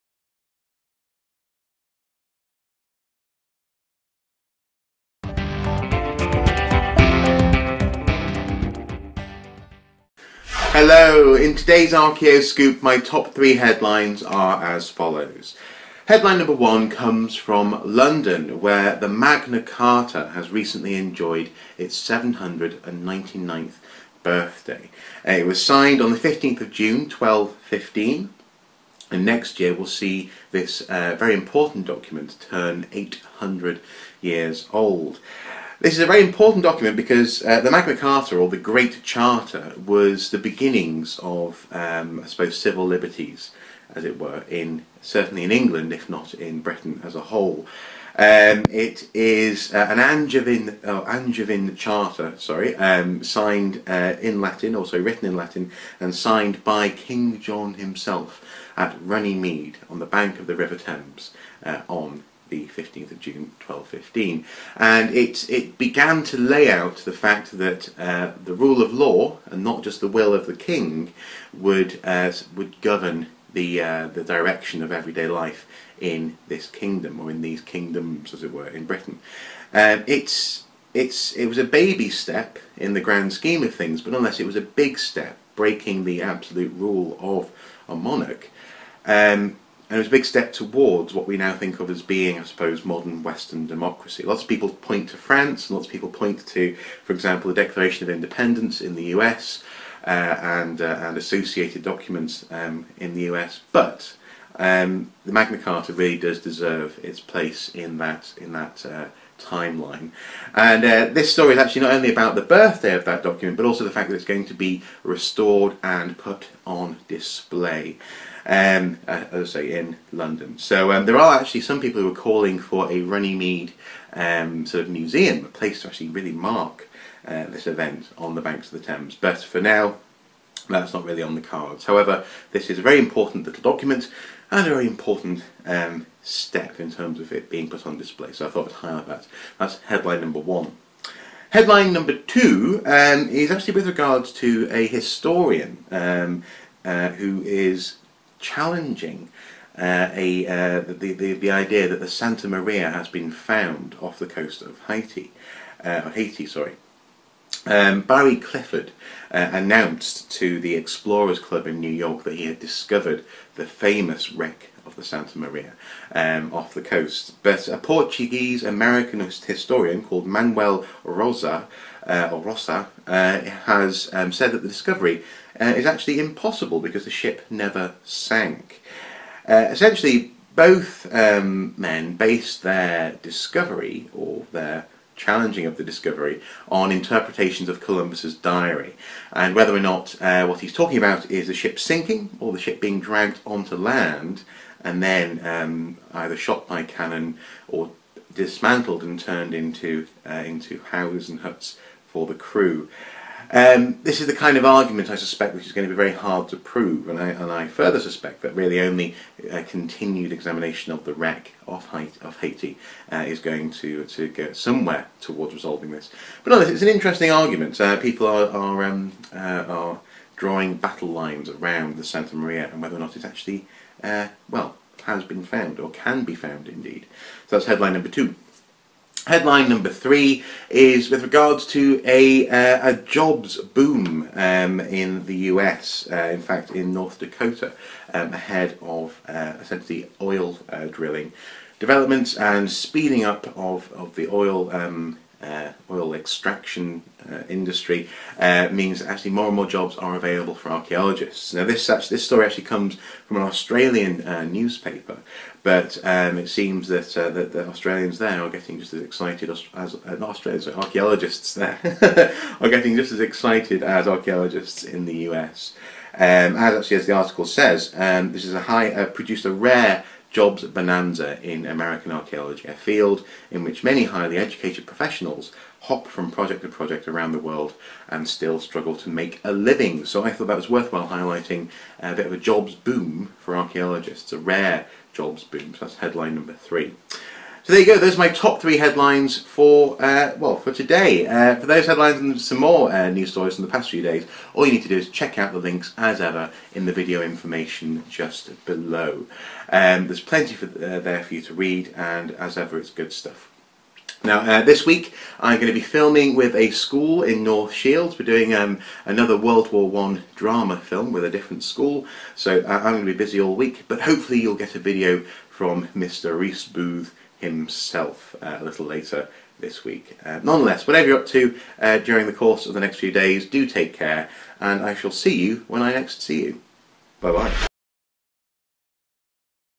Archaeoscoop: Magna Carta - 799th 'Birthday'
Welcome to Archaeoscoop, the place to find heritage and archaeology related news stories from around the world!

Today the Magna Carta is 799! A Historian casts doubt over the Santa Maria and there seems to be a jobs boom coming for Archaeologists:

1) Magna Carta, the cornerstone of English Law and Liberty to be restored and displayed:

Archaeologists find ketchup, fishy condiments, beer and radio remains at World War II Prisoner of War Camp: